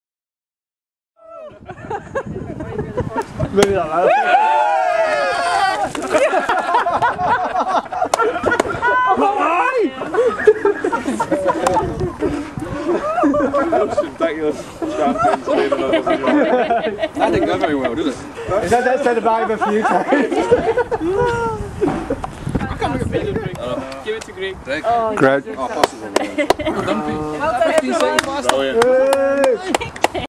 A Wet Champagne Spray
How not to open a bottle of champagne without getting soaked